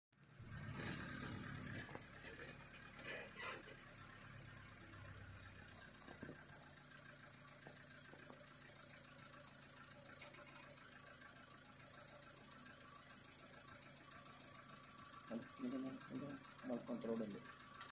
Leopard near camp area # short
Assam Manas area midnight one leopard on road,